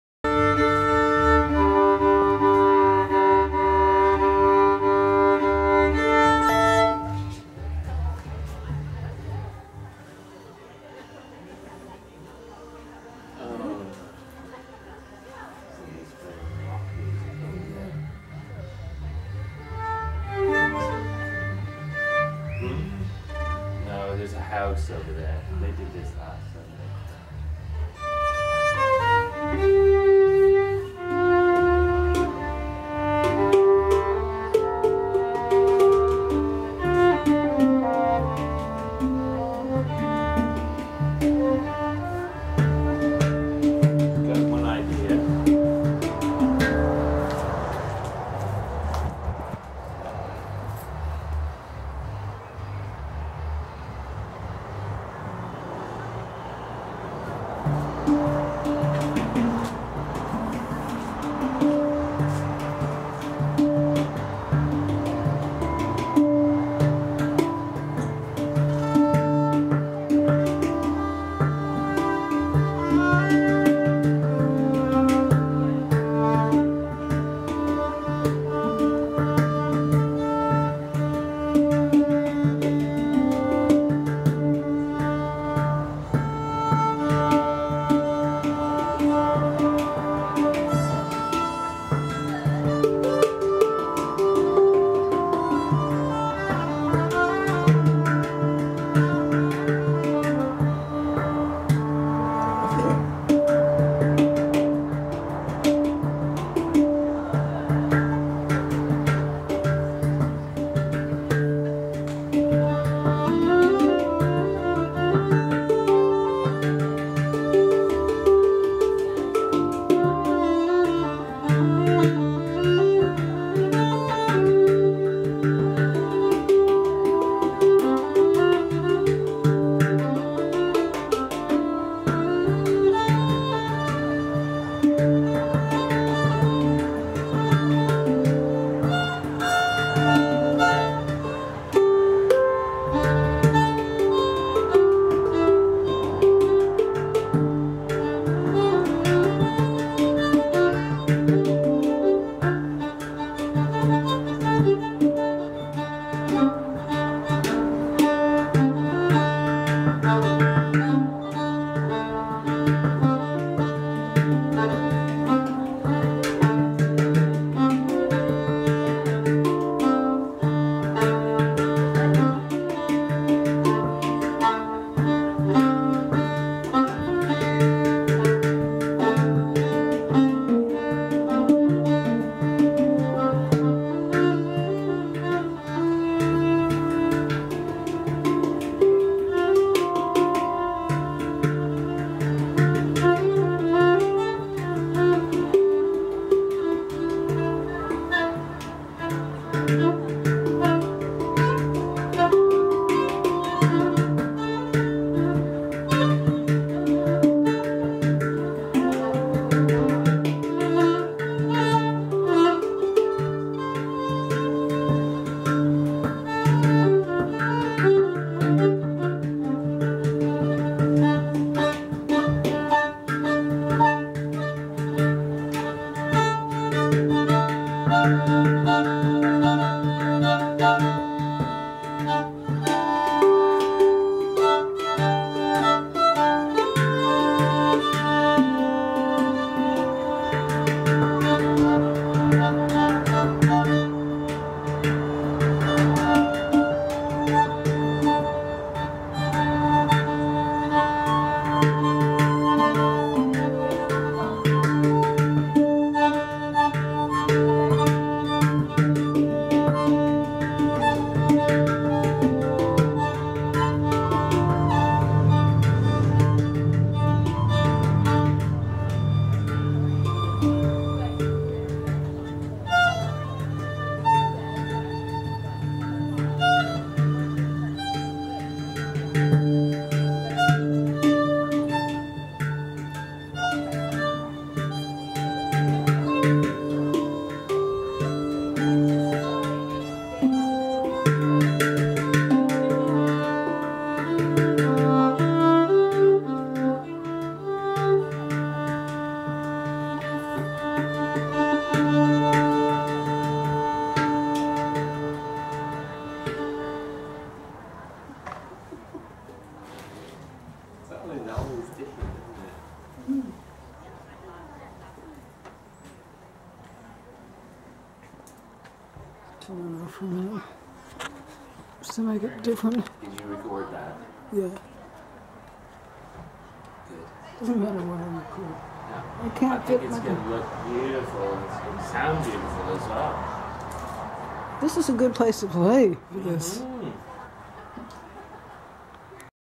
Fiddler Dave and CarolW at Shirley Country Market - a
At Pioneer Park, Shirley, B.C., in the Jim Snow Pavilion during the
Sunday Country Market. Kurd9 SPb Pantam My filename - MVI 1656
Dedicated to Pony